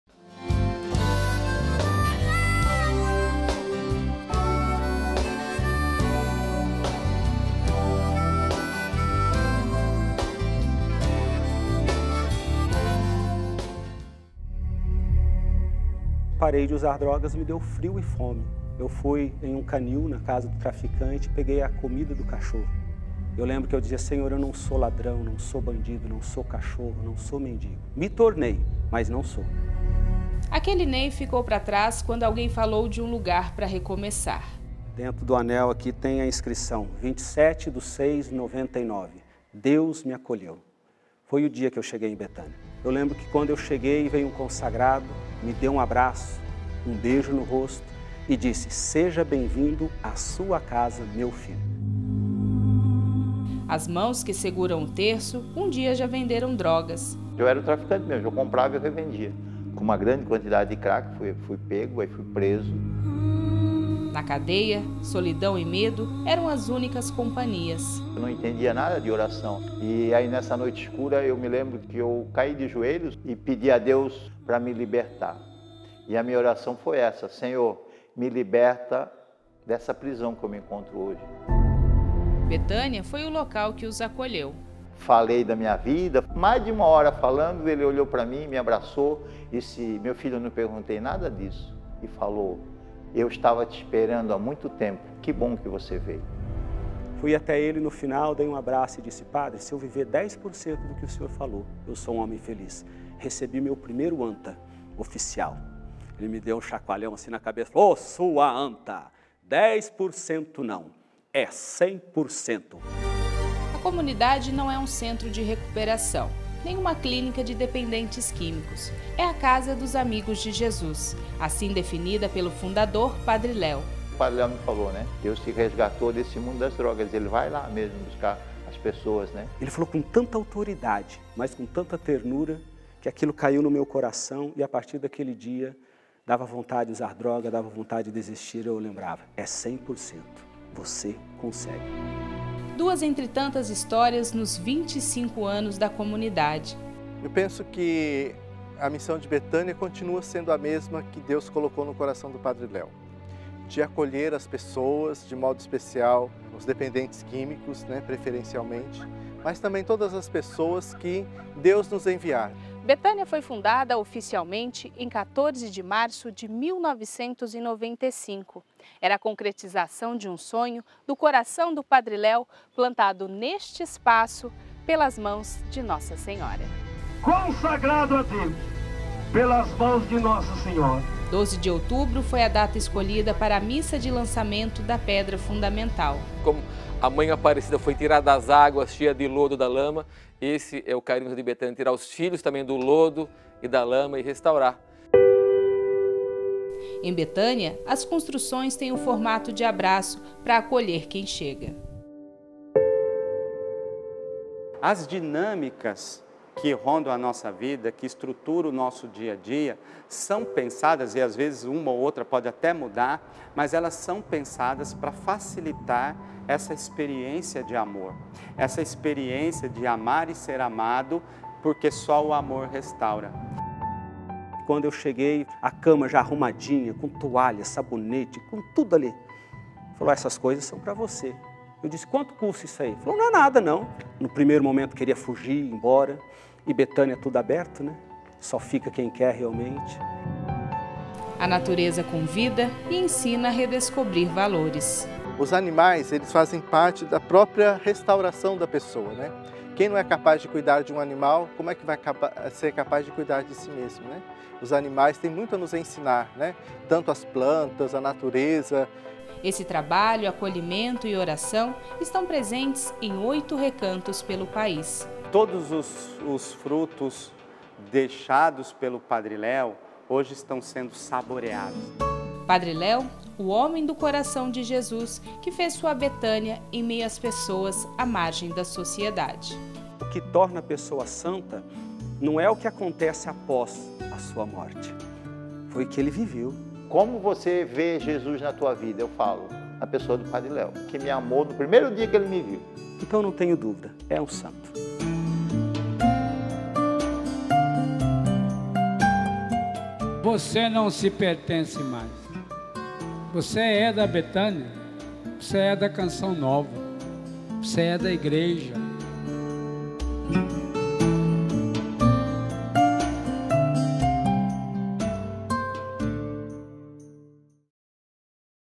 O maior legado deixado por Padre Léo: a Comunidade Bethânia
Bethânia, comunidade que acolhe pessoas à margem da sociedade, é o maior legado de Padre Léo. Esse trabalho, em 2020, completa 25 anos. E para marcar este momento, será realizada a abertura do processo de beatificação do fundador.

Reportagem de Elaine Santos, Messias Junqueira e Ederaldo Paulini